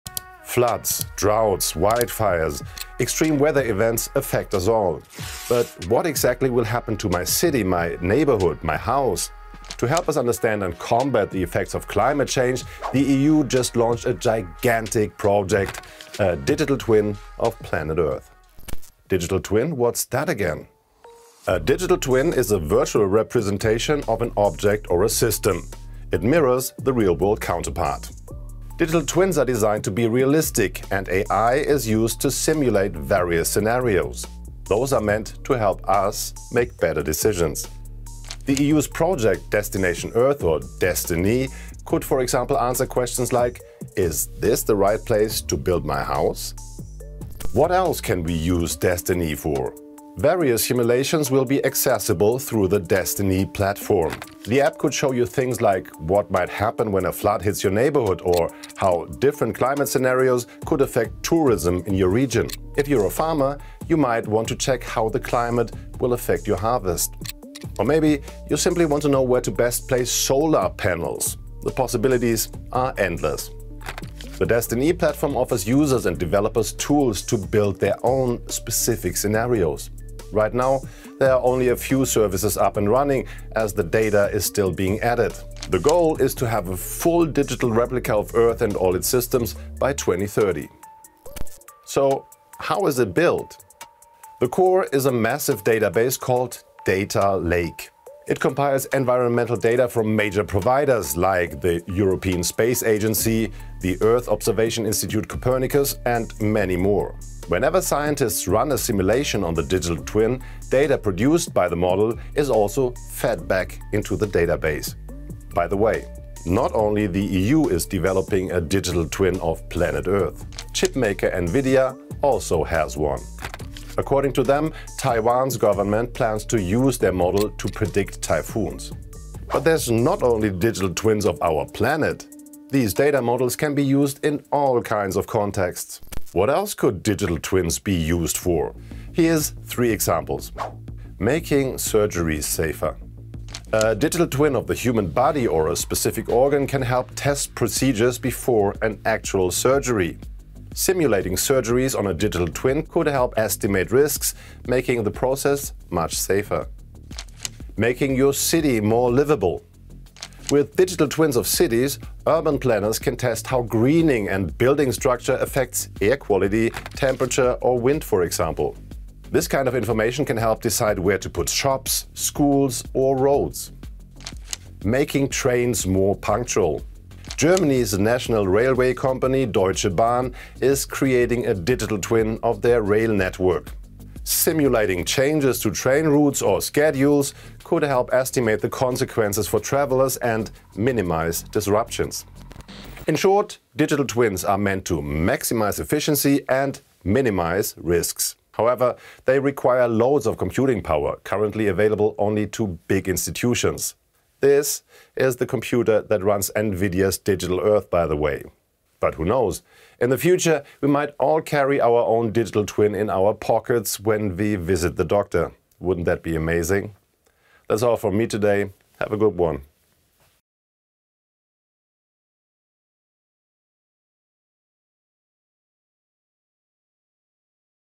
Can Earth Be Saved by Its Digital Twin?
Floods, droughts, wildfires - extreme weather events affect us all! But what exactly will happen to my city or my house? To help us understand, the EU is creating a digital twin of the earth.